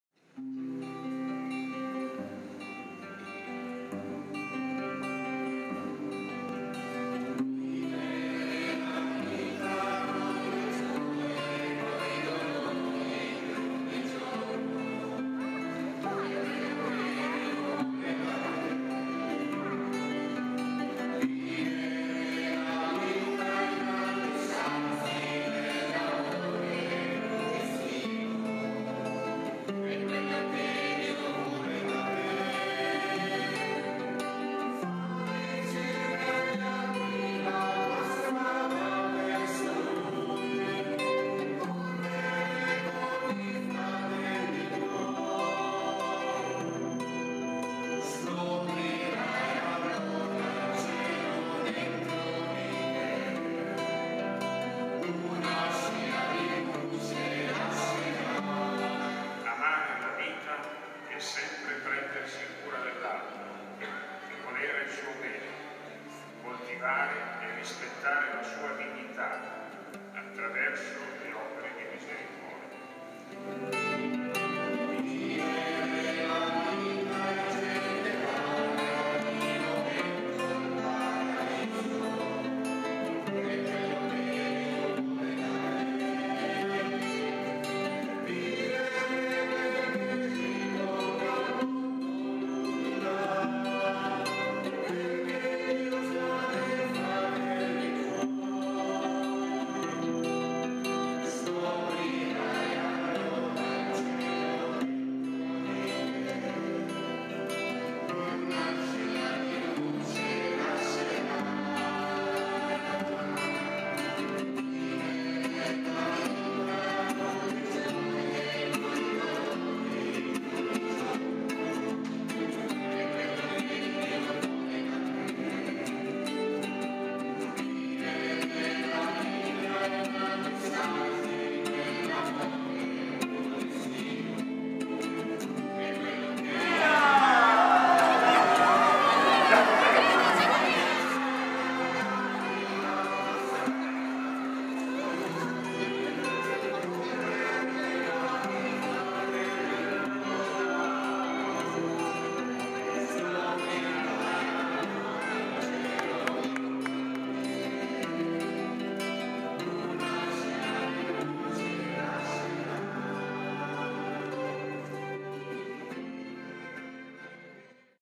Festa della Vita 2016
Palloncini colorati, palloncini fortunati: quest’anno 2016 è proprio stato così. Tanti i palloncini colorati gonfiati da Francesco per fare festa alla vita. Da un calcolo sommario circa trecento! Una bella tradizione di ogni anno. E nonostante tante influenze, numerosi i bambini presenti per dire grazie a Maria, la mamma della vita. Presenti anche numerose famiglie dei bimbi battezzati nel 2015, invitati espressamente per la celebrazione. All’offertorio, insieme ai doni per l’Eucaristia, sono stati offerti anche un palloncino giallo ed una primula della vita. Il Signore unisce infatti al suo sacrificio la vita dei suoi figli e fedeli. Al termine della messa è poi molto partecipata la preghiera davanti all'immagine della Madonna delle grazie: preghiera per tutti i bambini perché “crescano buoni e santi”.